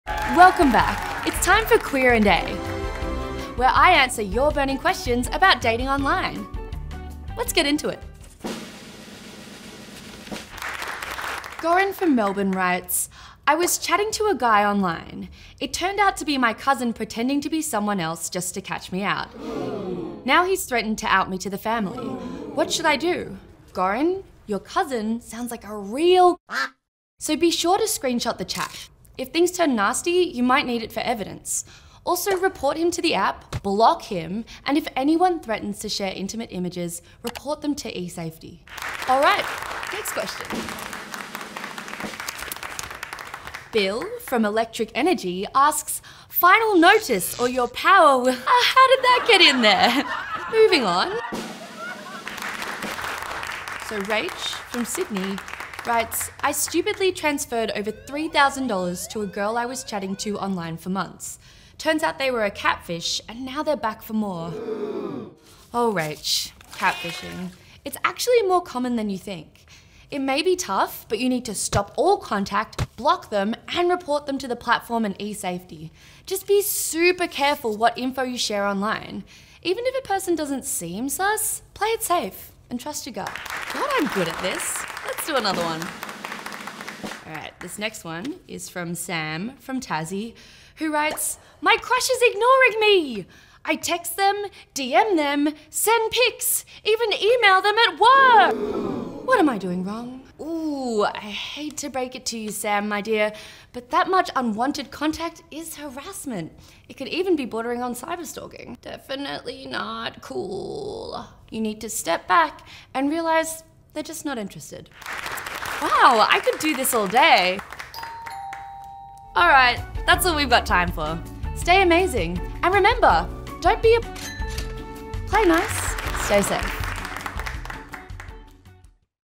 Queer & A - LGBTIQ+ online dating dos and dont's
Nina Oyama answers your questions about online dating and what you can do to avoid situations such as catfishing and cyberstalking. Find out how you can play nice and stay safe online by exploring a range of topics designed for the LGBTIQ+ community, with eSafety's LGBTIQ+ Learning Lounge